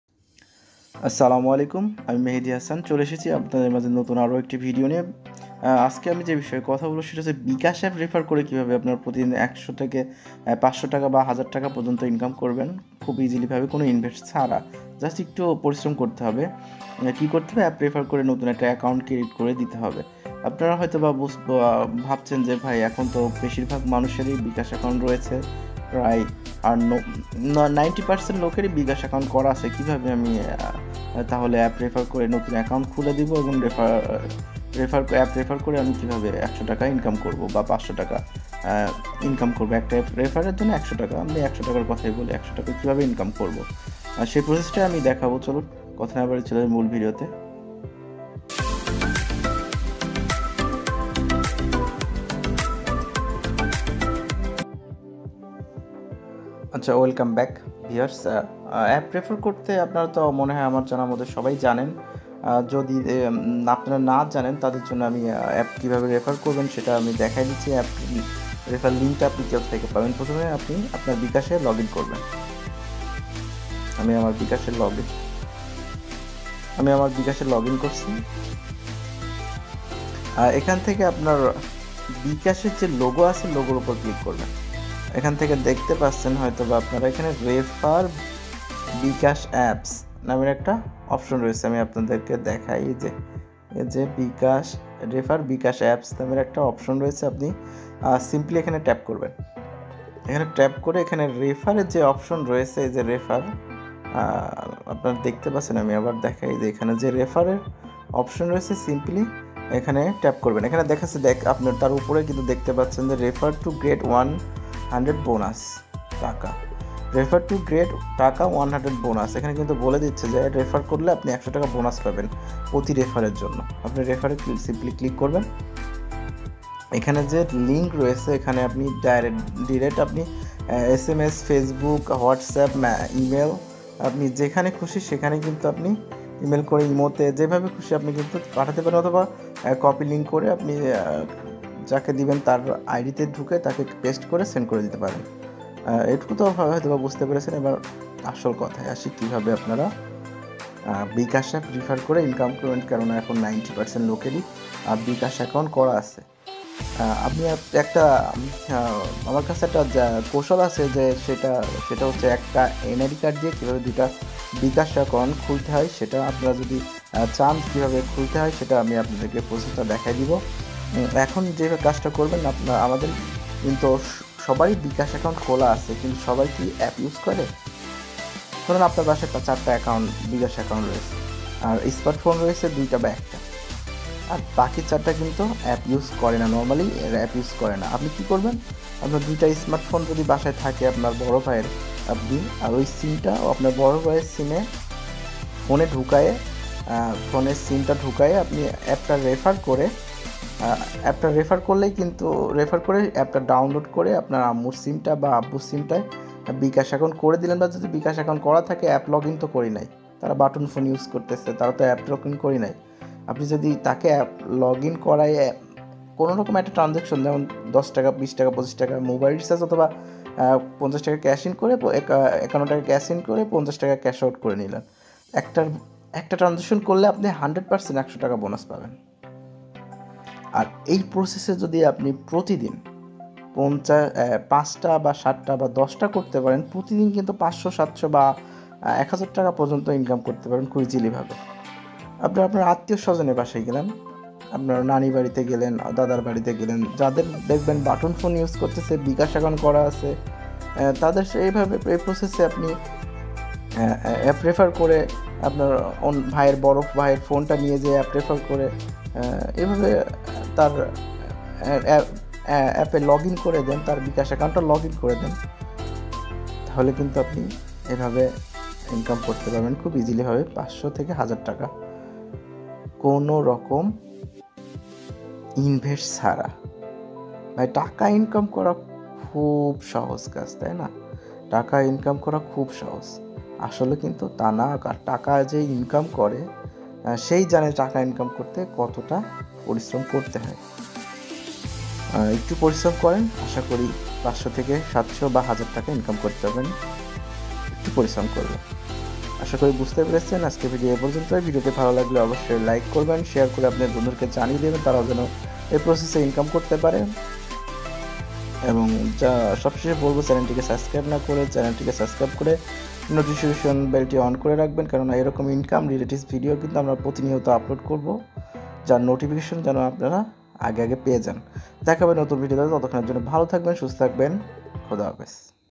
প্রতিদিন ৪০০ থেকে ৮০০ টাকা আয় করুন একদম ফ্রী তে
শুধু মাত্র অ্যাপ রেফার করে পতিদিন আয় করুন